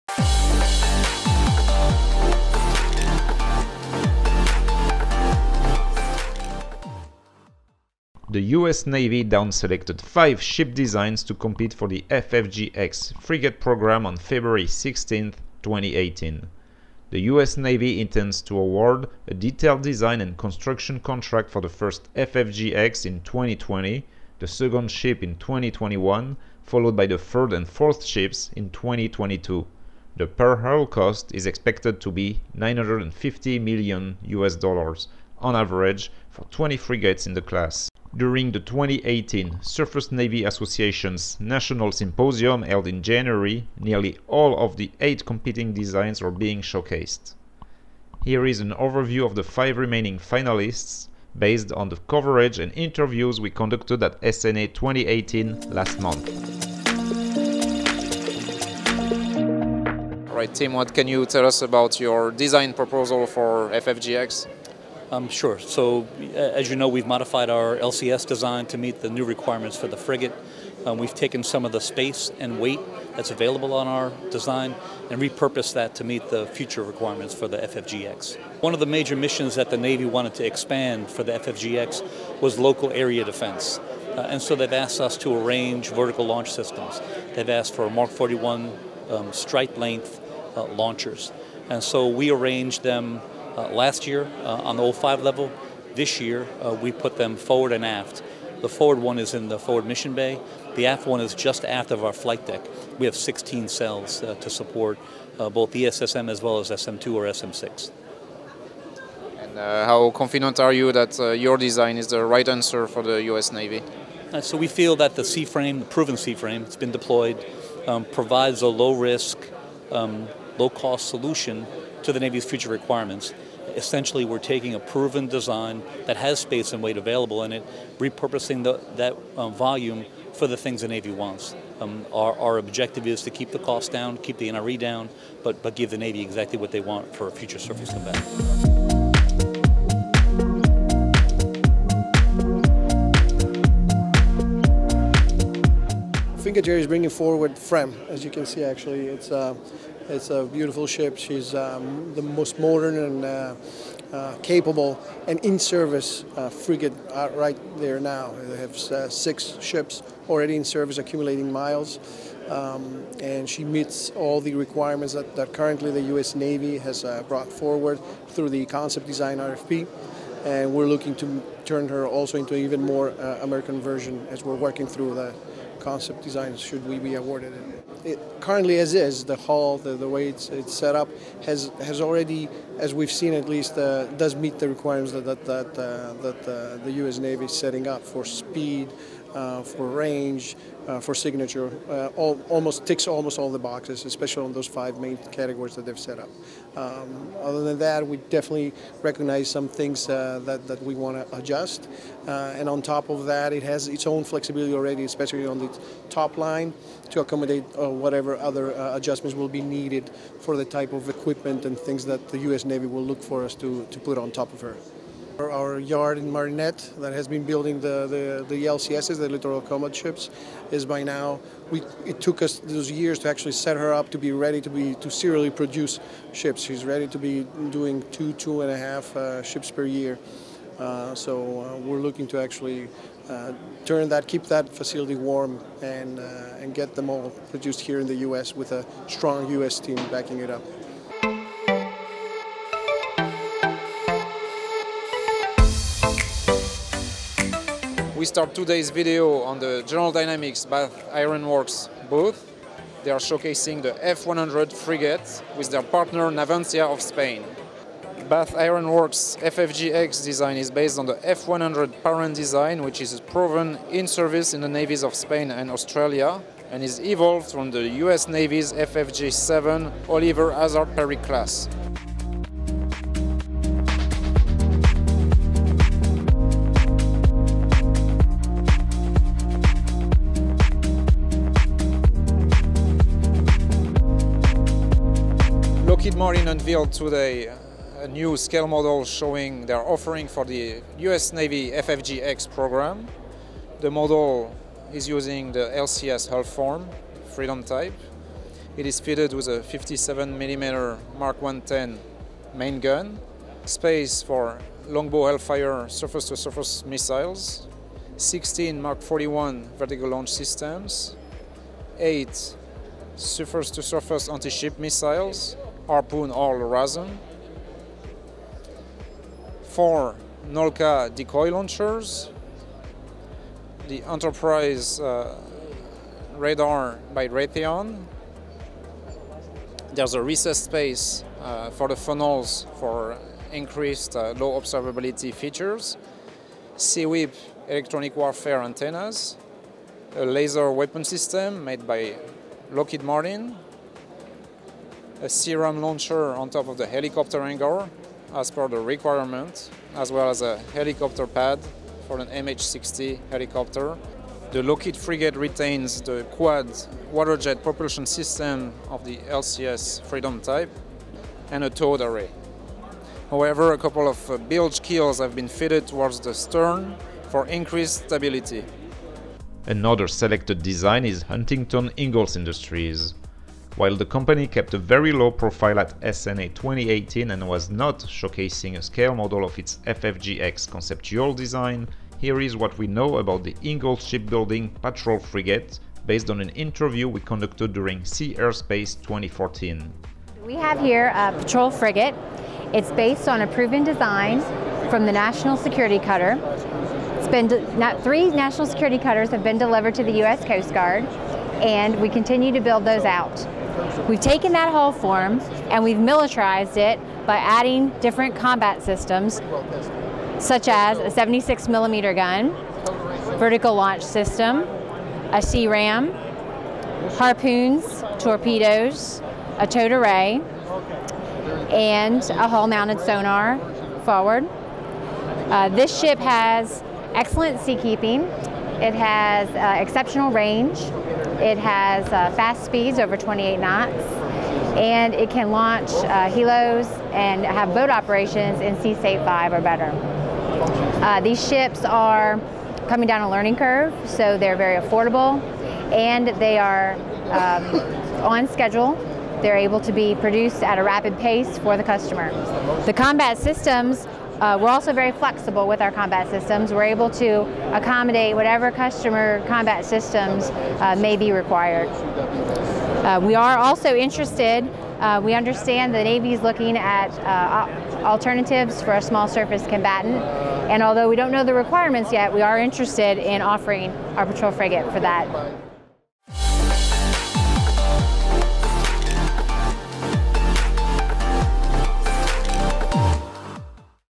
The Five Finalists for the U.S. Navy FFG(X) Next Generation Frigate Program
The U.S. Navy down selected five ship designs to compete for the FFG(X) Frigate Program on February 16, 2018. Austal USA, Fincantieri Marinette Marine, General Dynamics Bath Iron Works, Lockheed Martin and Huntington Ingalls Industries received a contract award for "conceptual design" of their frigate proposals.